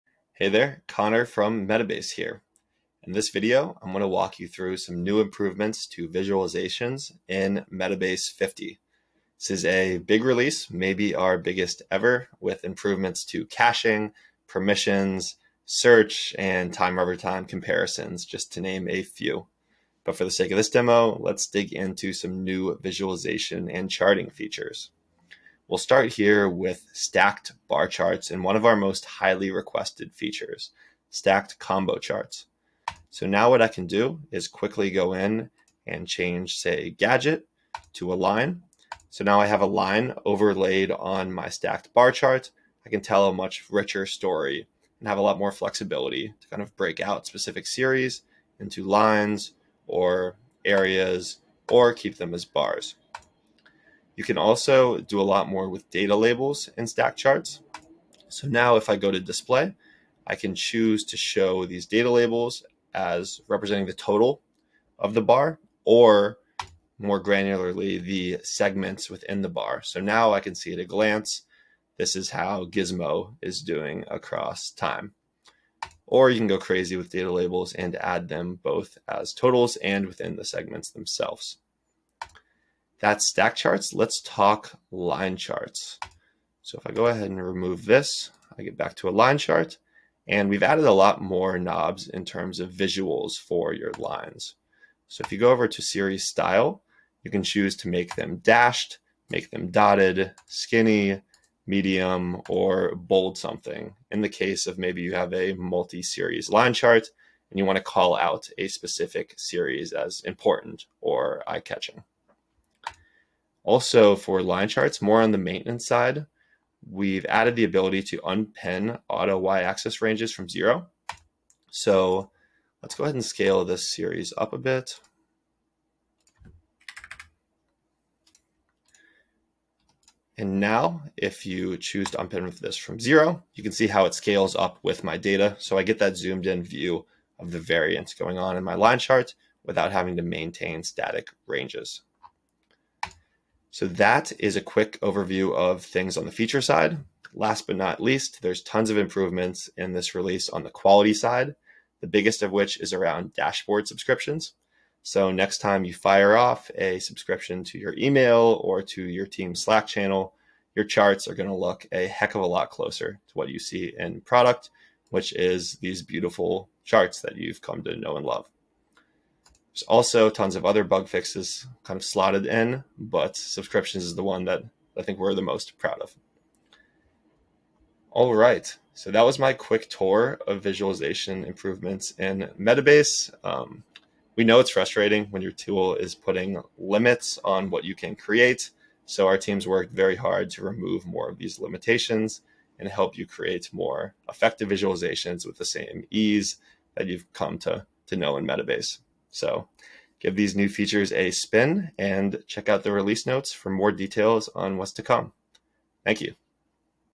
What's new in Metabase 50: Charting Improvement 🎉
The Metabase 50 release contains lots of big improvements to caching, permissions, navigation and more.

But the improvements to visualizations probably impact the broadest range of people using Metabase, giving you more ways to get your charts to looking like you want, while also making them easier for everyone to interpret.

Check out Product Manager, Conor Dewey's walkthrough of the new visualization features. Try them out for yourself in Metabase and let us know what you think!